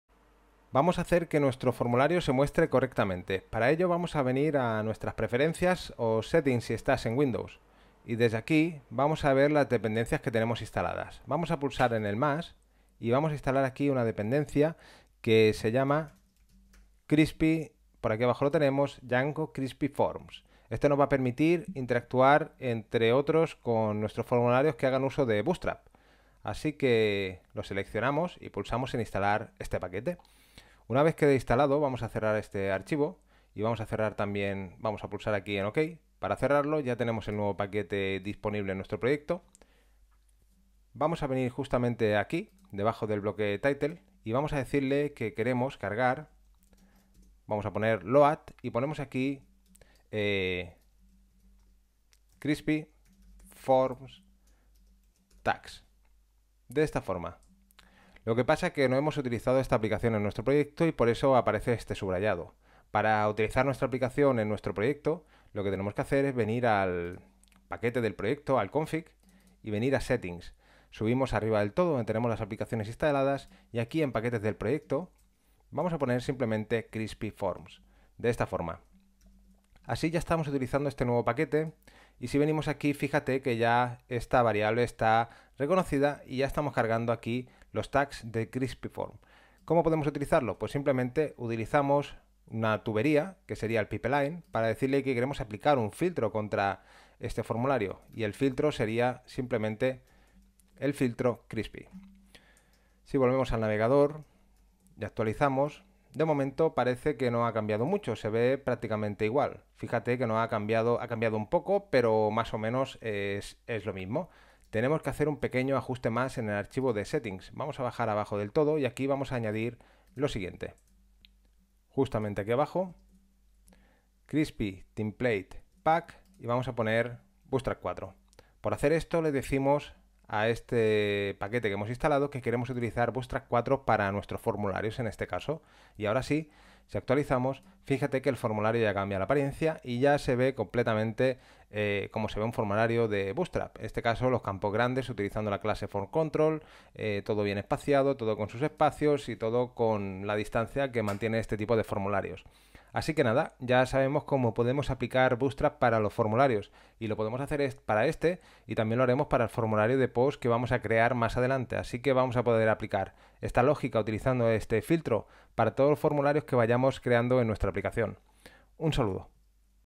Curso de Django: Instalar Crispy Forms para utilizar formularios con Bootstrap 4
Bienvenid@ al curso de Django, en este vídeo aprendemos a instalar el paquete Crispy Forms con Pip para darle una apariencia más agradable a nuestros formularios.

🔥Accede al curso y descarga el proyecto final en Cursosdesarrolloweb🔥